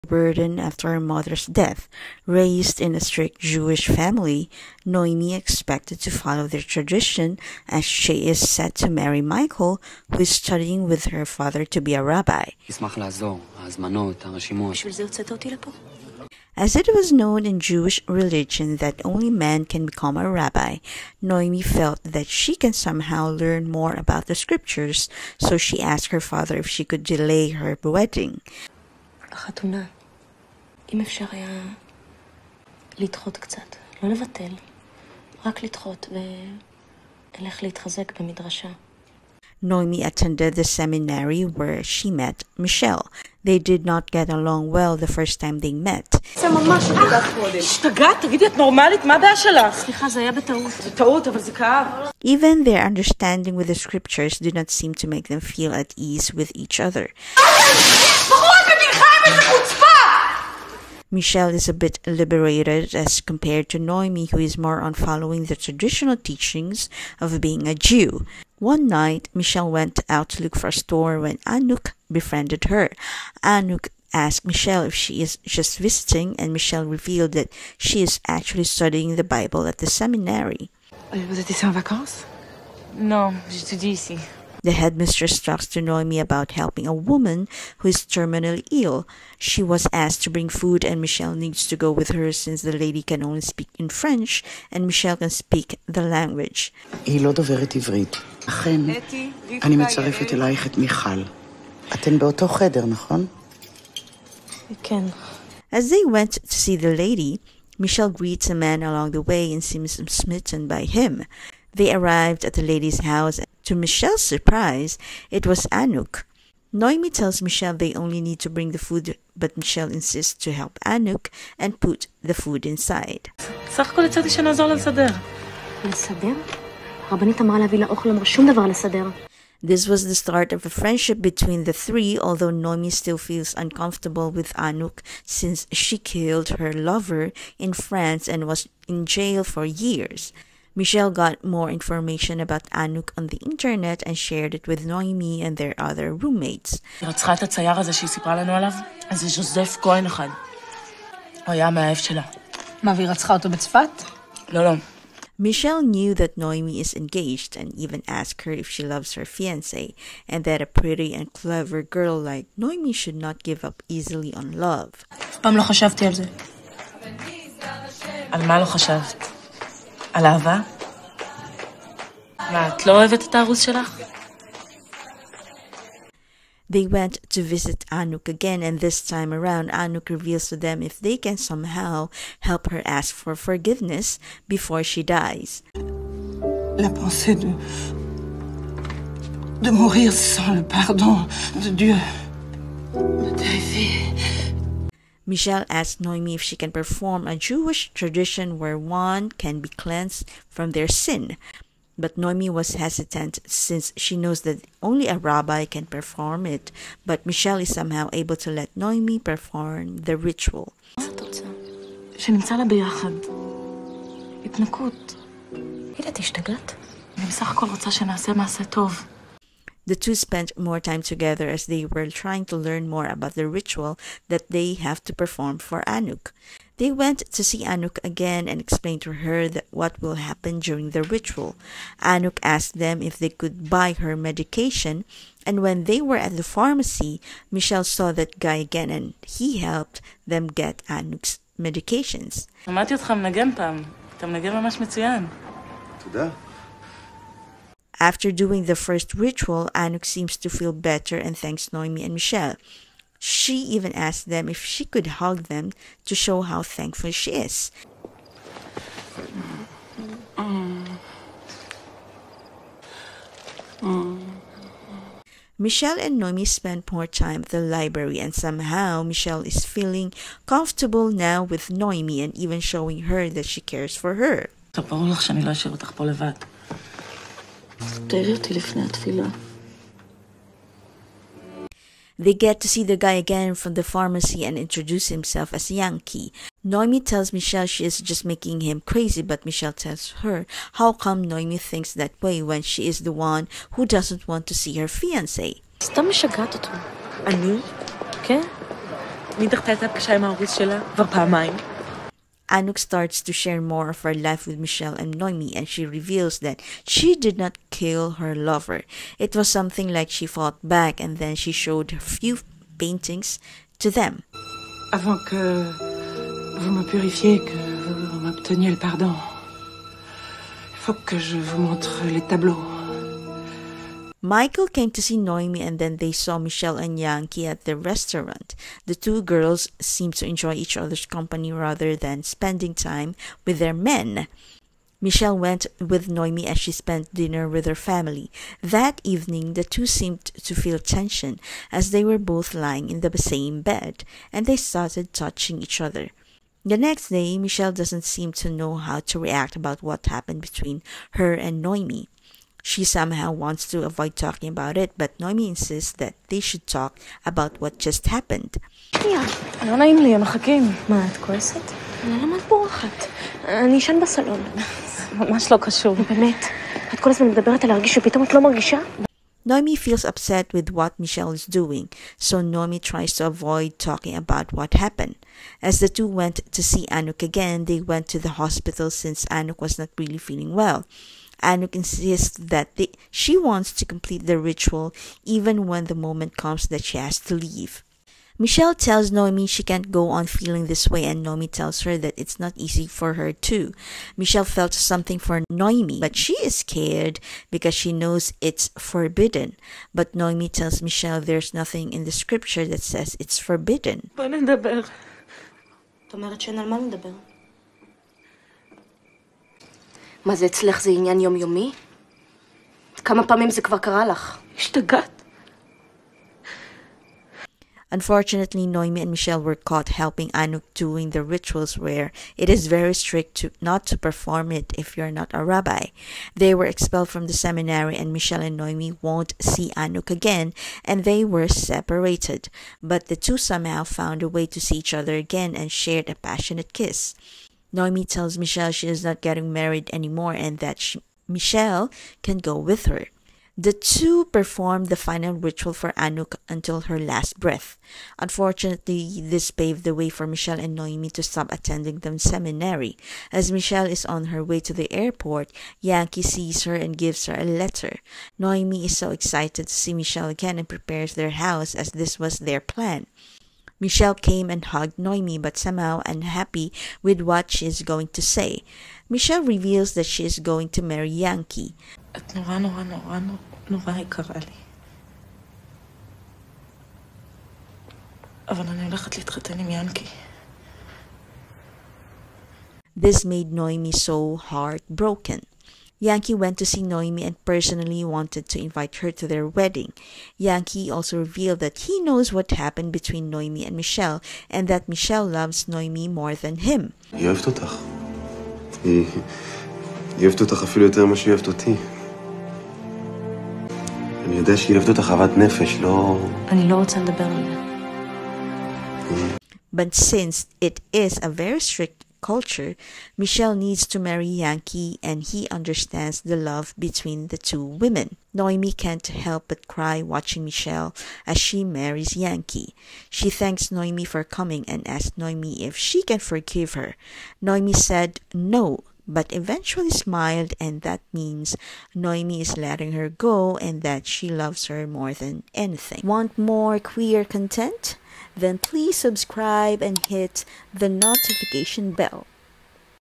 They FELL INLOVE inside the seminary while helping a Dying prisoner!
Noemi feels the burden after her mother's death. Raised in a strict Jewish family, Noemi is expected to follow their tradition as she is set to marry Michael who is studying with her father to be a rabbi.
As it was known in Jewish religion that only man can become a rabbi, Noemi felt that she can somehow learn more about the scriptures so she asked her father if she could delay her wedding.

Noemi attended the seminary where she met Michelle. They did not get along well the first time they met. Even their understanding with the scriptures did not seem to make them feel at ease with each other.